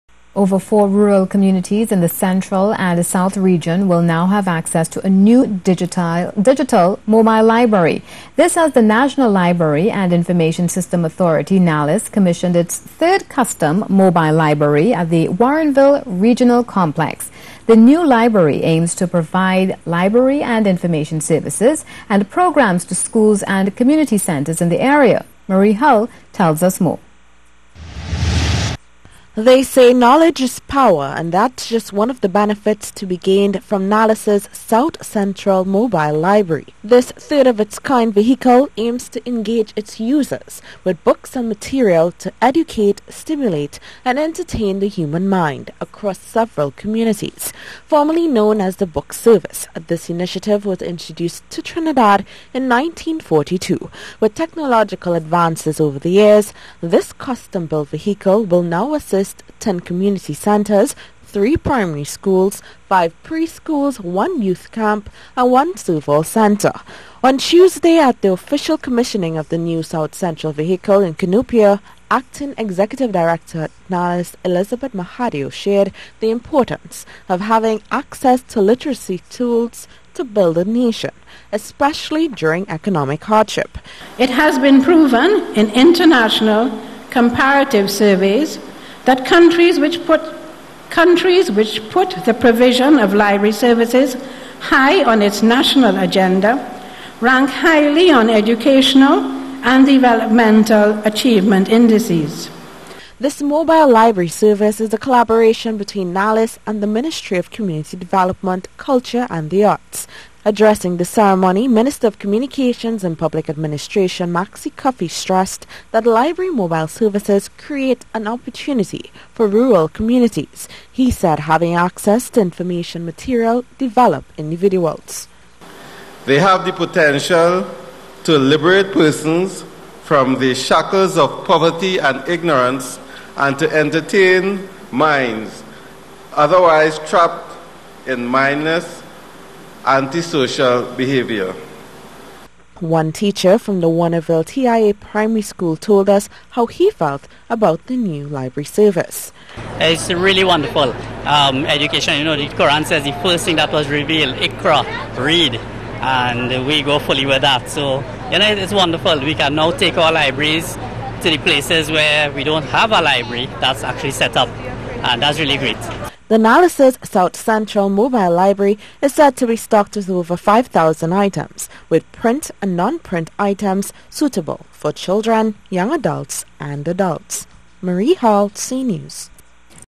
NEW NALIS MOBILE LIBRARY:
Over four rural communities in the Central and South region will now have access to a new digital mobile library. This as the National Library and Information System Authority (NALIS) commissioned its third custom mobile library at the Warrenville Regional Complex. This new mobile library aims to provide library and information services and programmes to schools and community centres in the area. Marie Hull tells us more.